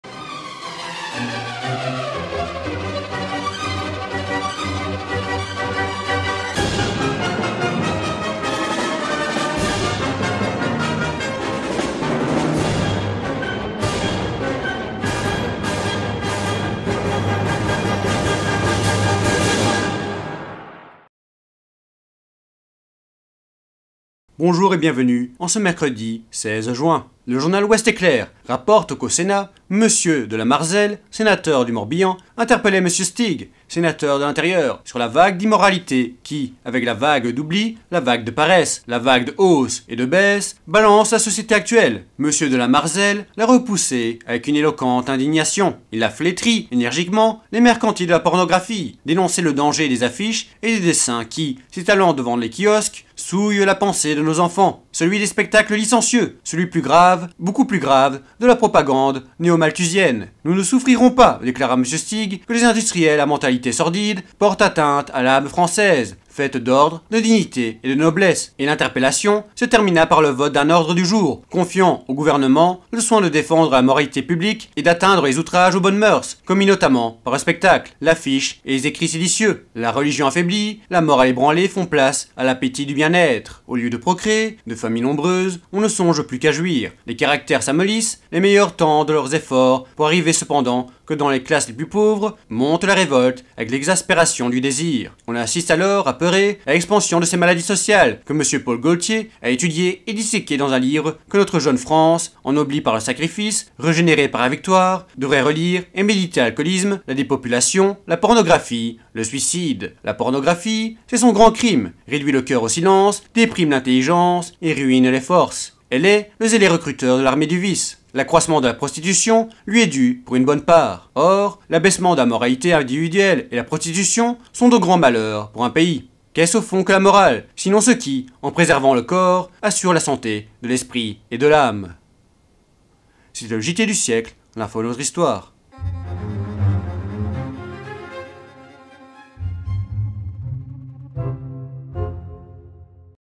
JTS 1920 06 16 Contre la vague d'immoralité
L'actu la plus récente d’il y a cent ans jour pour jour
C'est le JT du Siècle, l'info de notre histoire

Jeudi 17 juin 1920

LA PREMIÈRE INDÉCISION de la Société des Nations

La Société des Nations s'efforce de prouver son existence. Dans la journée d'hier, à La Haye, M. Léon Bourgeois a installé un comité de juristes chargés d'organiser la Cour permanente de justice internationale, « dont les sentences devront garantir le maintien de la paix. En même temps, à Londres, lord Curzon formulait la décision du Conseil directeur de la Société au sujet du premier litige dont celle-ci ait été saisie.
Il s'agissait de la Perse, dont le ministre des affaires étrangères, le prince Firouz, a sollicité l'intervention des Alliés contre les bolcheviks. Le cas, comme nous l'avons déjà expliqué, était très délicat et pour de nombreuses raisons. D'abord, il est très douteux que la nation persane souhaite qu'en intervenant dans ses affaires l'Entente sanctionne et renforce le protectorat que le gouvernement britannique prétend lui imposer. En second lieu, le cabinet de Téhéran, dont le prince Firouz se dit le porte-parole, tout en faisant appel à la Société des Nations, a concurremment, ou plutôt contradictoirement, entamé des négociations directes avec les bolcheviks.
Dans ces conditions ils ont décidé... de ne rien décider. La déclaration lue par lord Curzon, au nom du Conseil de la Société des Nations, motive par des « attendus » imposants cette très simple conclusion : « II faut attendre, » to wait and to see : voir venir les événements est une dès maximes favorites de la diplomatie britannique ; mais le Conseil de la Société des Nations, en l'appliquant, ne fait guère, preuve ni de franchise ni d'indépendance.

L'article 14 du pacte de la Société des Nations prévoit l'institution d'une Cour permanente de justice internationale chargée de juger tous les litiges qui pourront lui être; soumis, soit directement par les gouvernements intéressés, soit indirectement par le conseil de la Société. Un comité de dix éminents juristes a été chargé d'établir les statuts de cette cour permanente.
M. Léon Bourgeois a présidé la séance inaugurale du comité dans le palais de la Paix de La Haye et, dans un long discours, s'est efforcé de définir la tâche immense qui incombe aux créateurs du nouveau droit des gens.

Un tribunal, a-t-il dit, dont les sentences doivent garantir le maintien de la paix n'a d'autorité que si cette paix est fondée sur le droit lui-même. La paix, c'est la durée du droit. Dans toute la mesure où le droit aura été assuré par la victoire, vous pourrez, messieurs, travailler librement à son triomphe définitif.